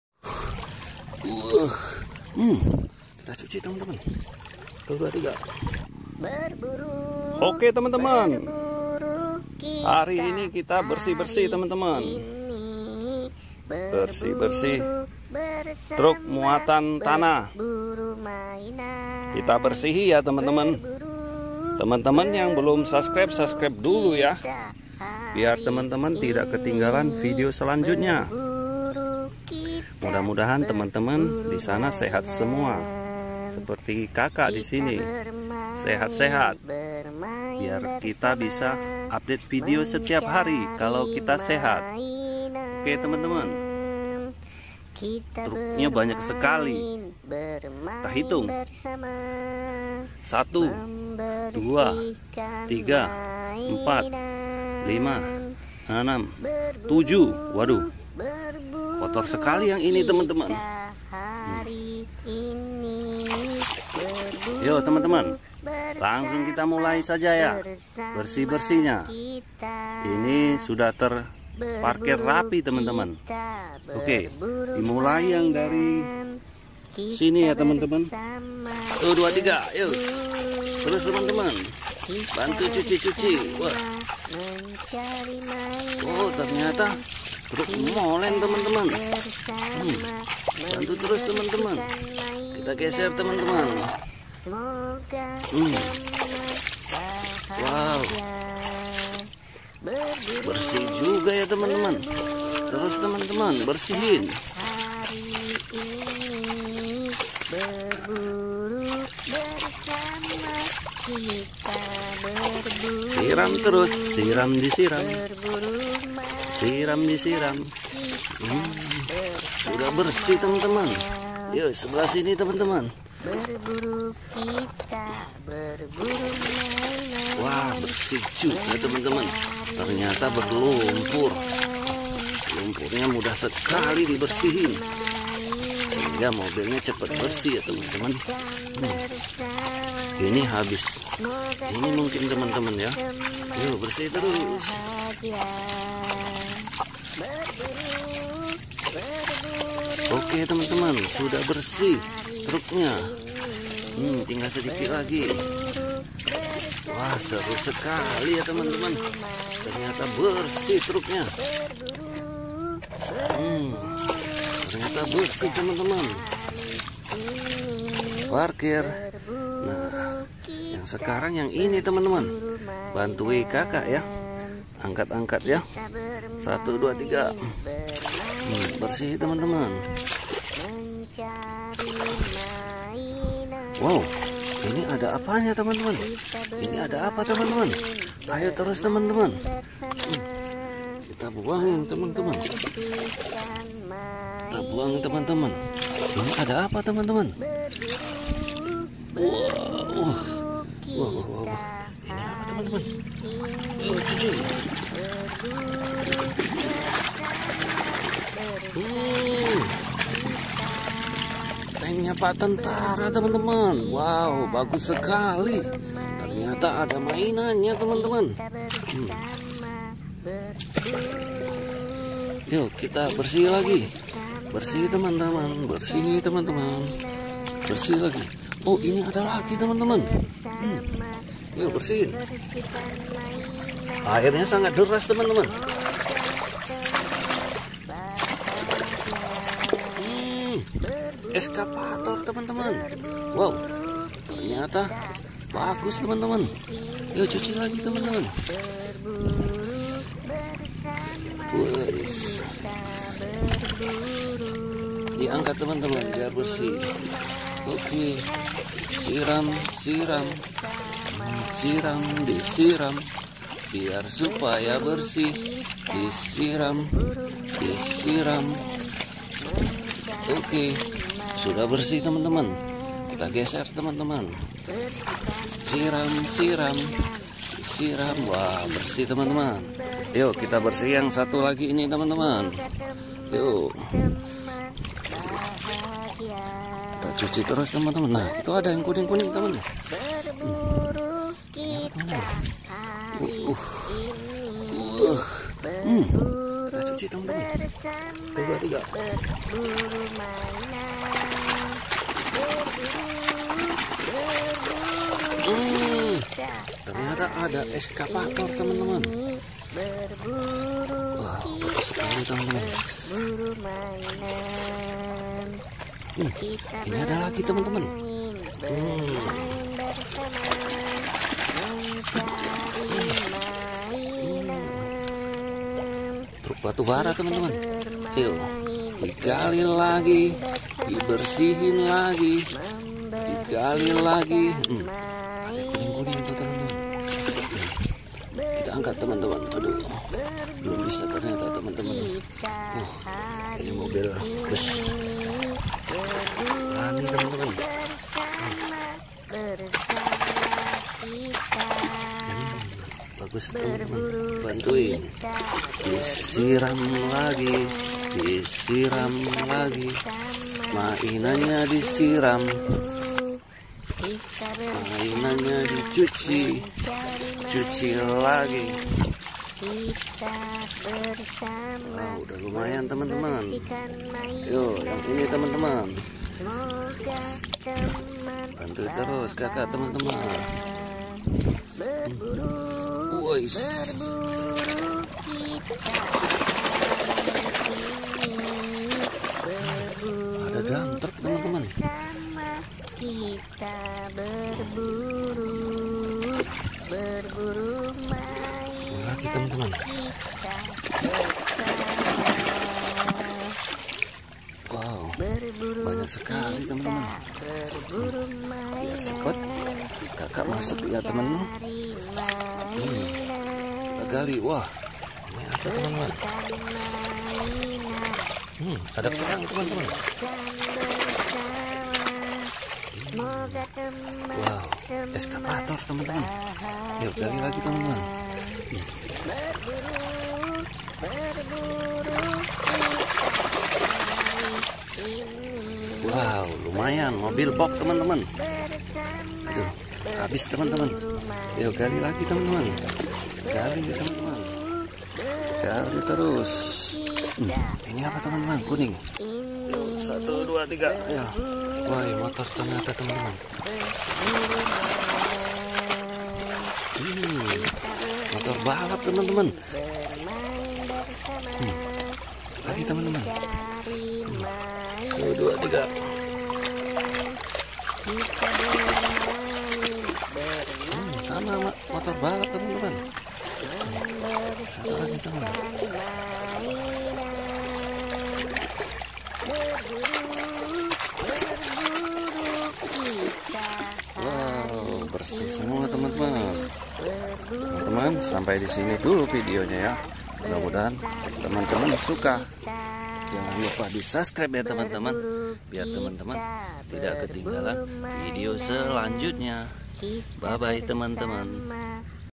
Membersihkan mainan mobil pasir, menemukan mainan ,mobil truk ,mobil molen, excavator
terimakasih by: @new toys truk